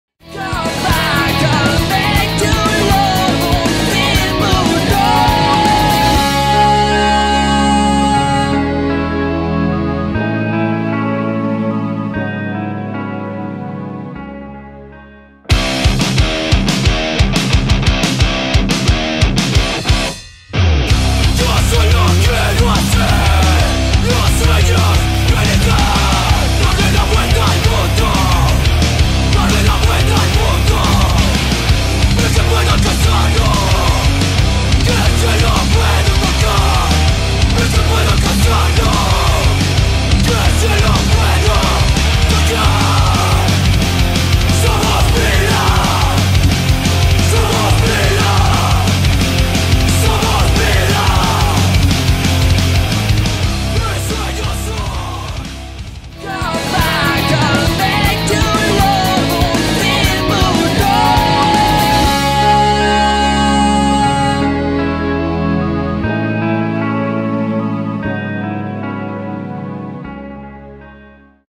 ClipZOOM Momentos Rock al Parque 20 años
Canal ZOOM estuvo presente en la fiesta del Rock, estos son algunos momentos del gran festival que acogió a mas de 400.000 personas en 3 días.